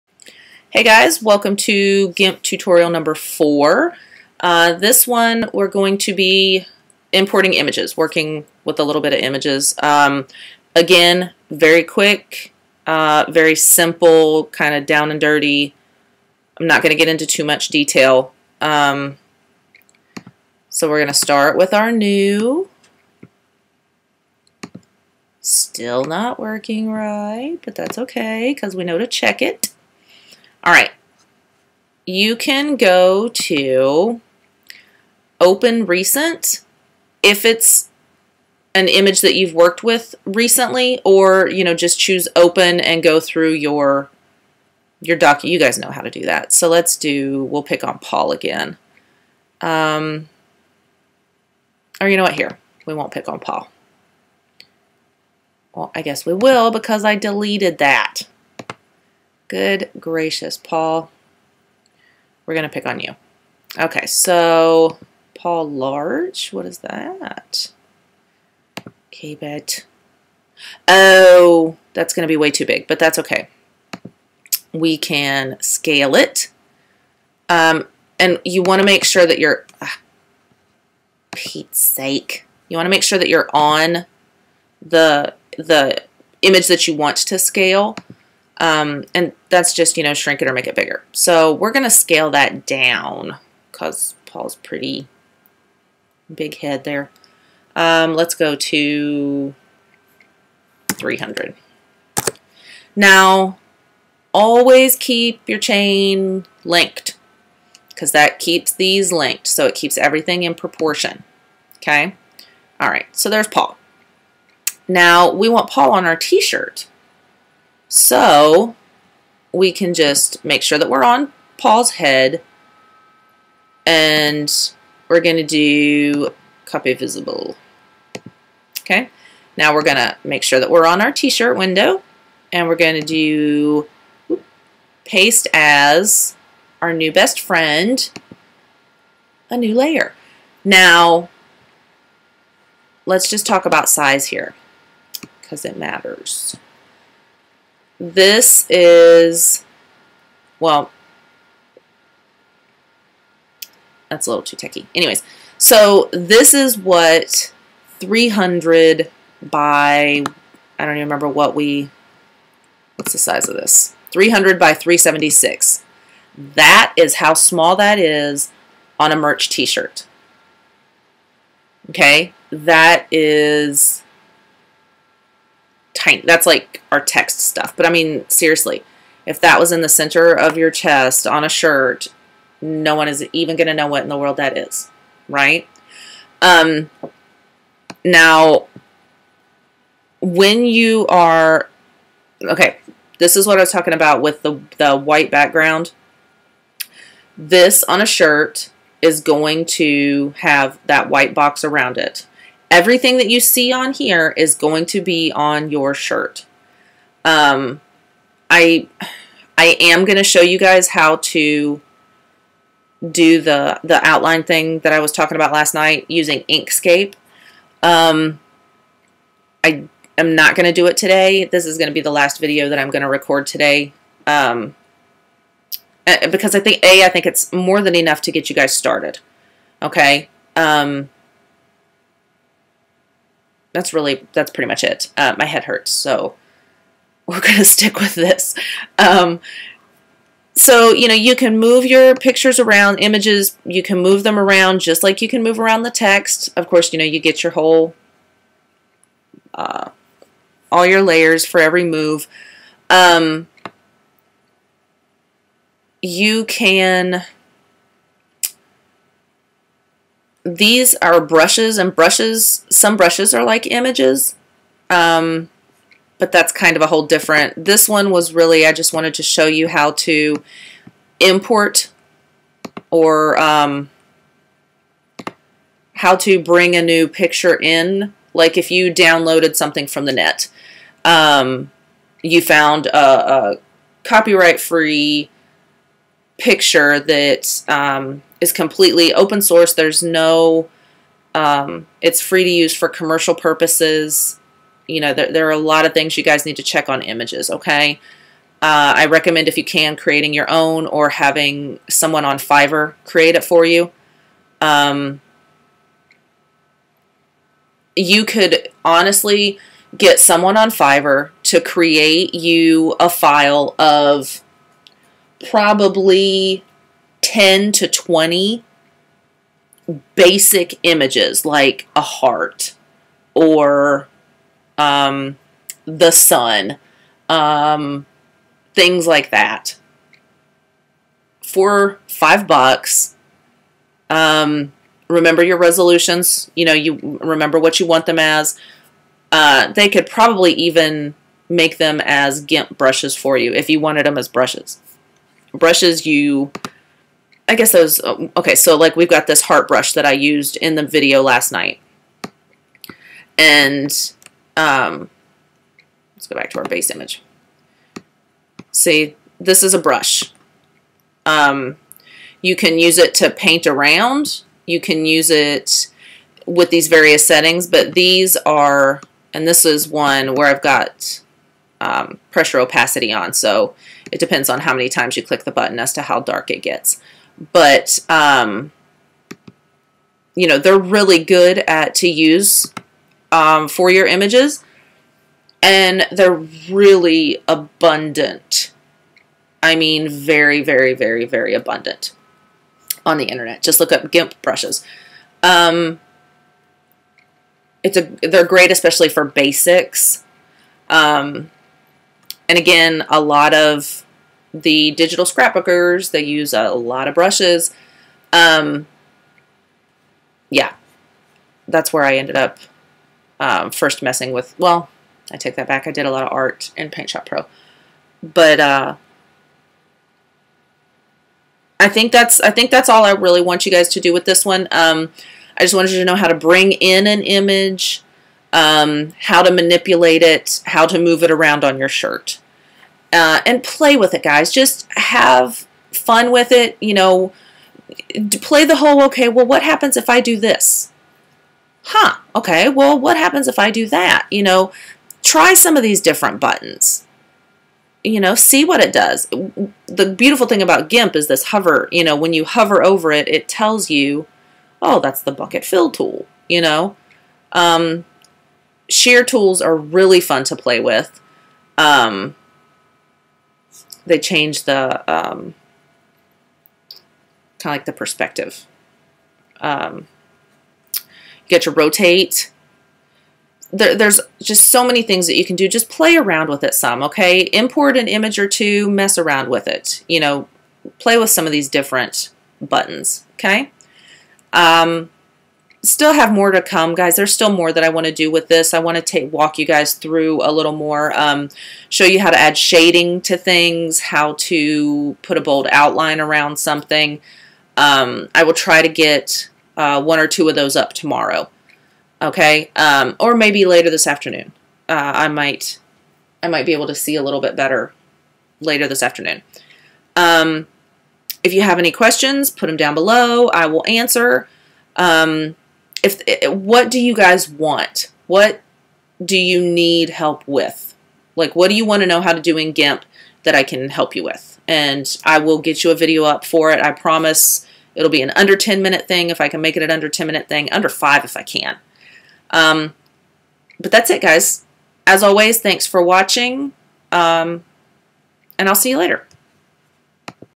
Gimp Tutorial for Merch #4 - Adding an Image
This video is showing you how to really easily bring in an image you've download or made to your Merch shirt template.

Please post any questions or suggestions in the comments.